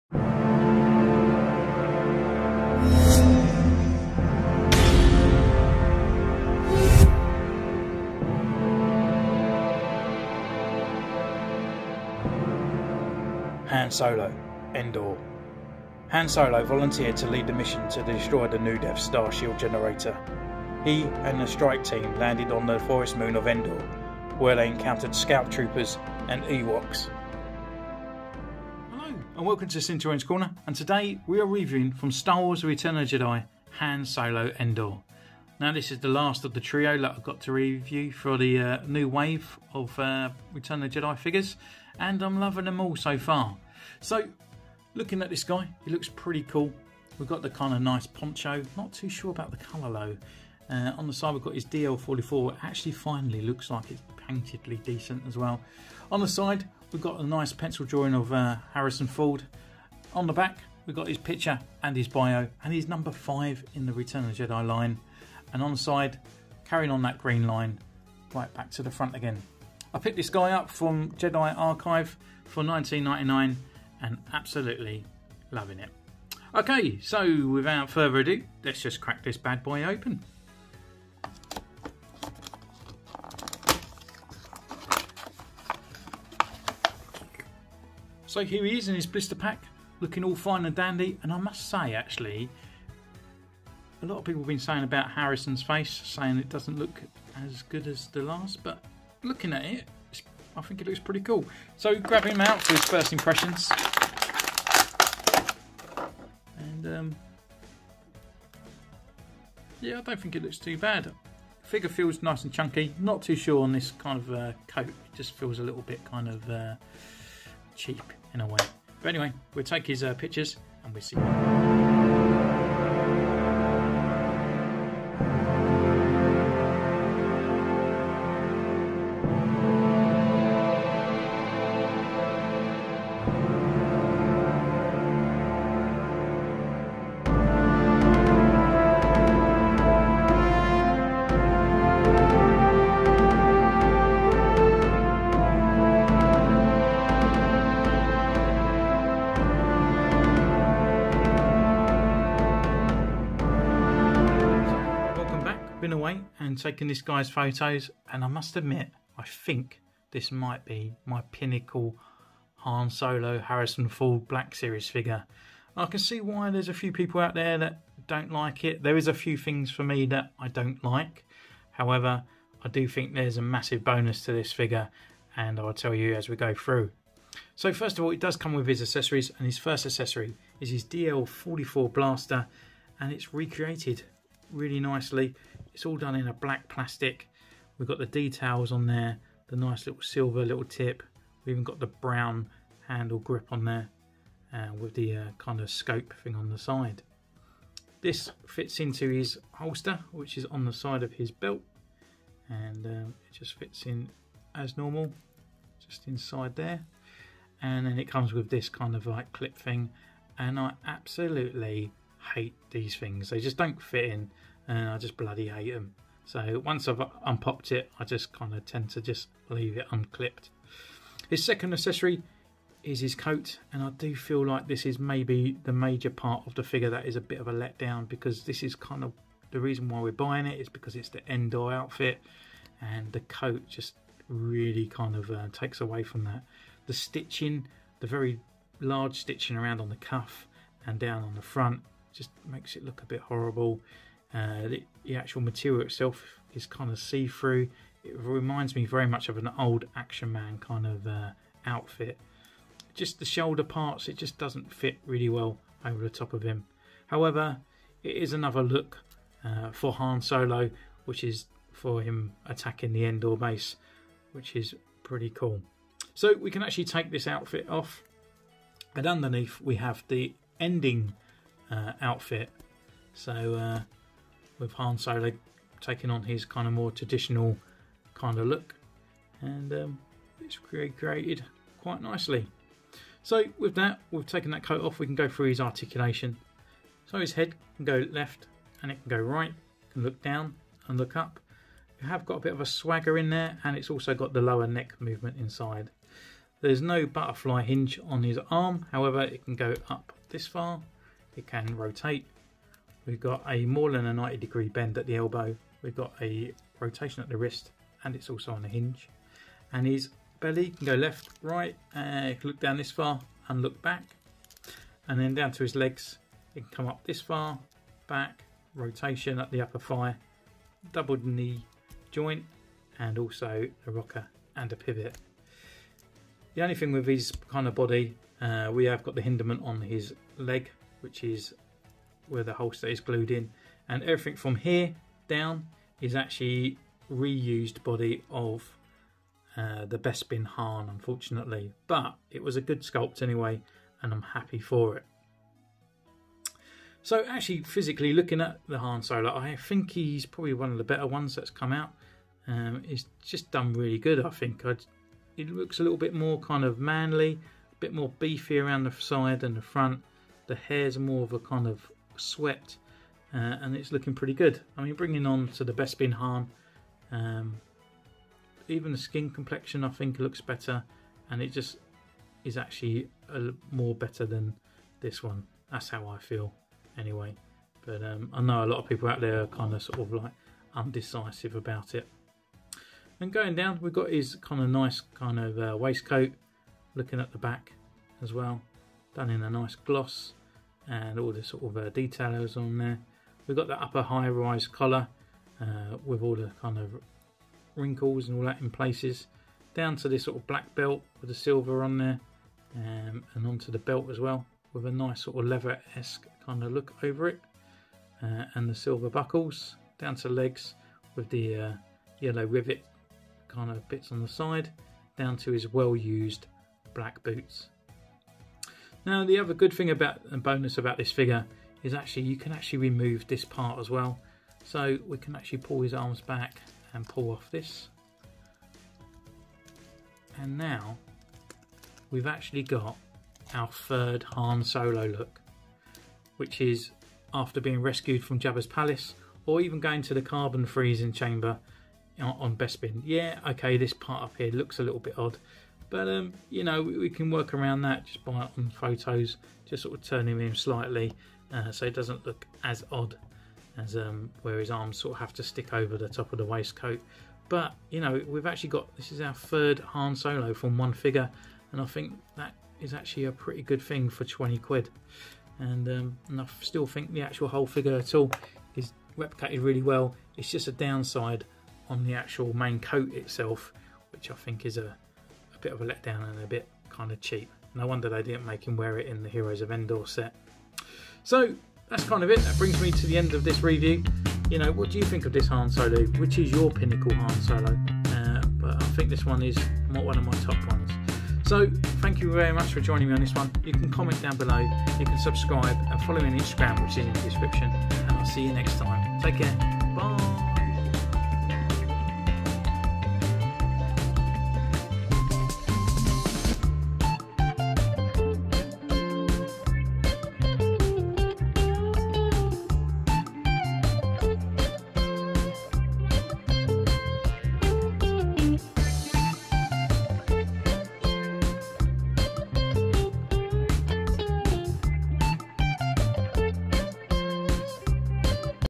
Star Wars The Black Series Princess Han Solo Endor 05 Return of the Jedi 6 Inch Action Figure Review
Hello

Welcome to Centurion's Corner
These roughly stand 6inchs tall or higher

Reviews, Customs, Tutorials, Live Chats